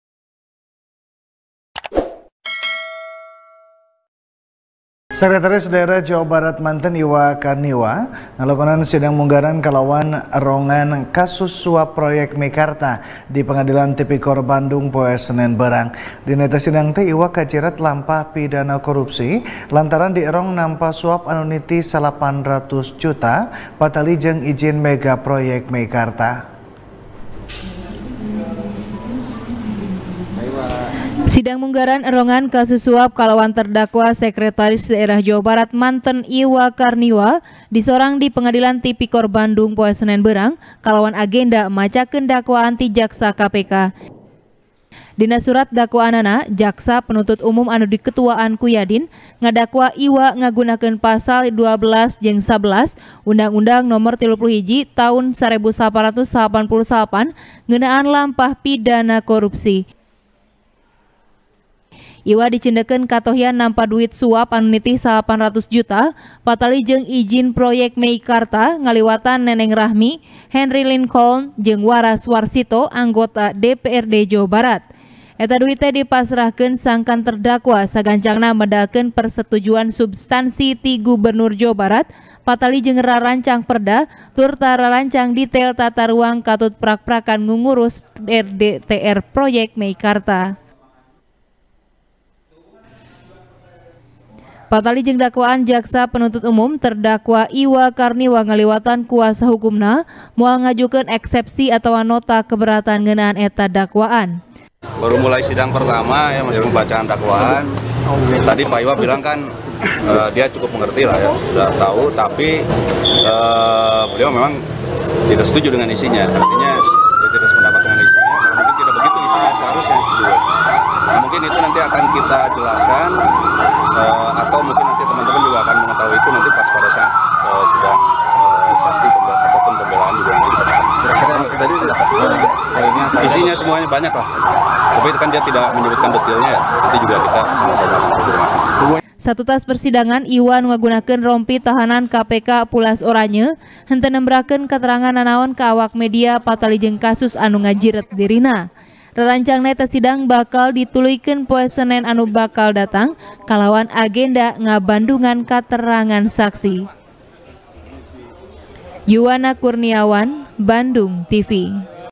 SEKDA JABAR Manten Iwa Karniwa Kaancem Hukuman 12 Taun Bui | TP | BANDUNGTV
TP | BANDUNGTV | DERI FIRMAN | Sekretaris Daerah Jawa Barat Manten, Iwa Karniwa, Ngalakonan Sidang Munggaran Kalawan Aerongan Kasus Suap Proyek Meikarta, Di Pengadilan Tipikor Bandung, Poe Senen Beurang. Dina Eta Sidang Teh, Iwa Kajiret Lampah Pidana Korupsi, Lantaran Dierong Nampa Suap Anu Nitih Salapan Ratus Juta, Patali Jeung Ijin Mega Proyek Meikarta.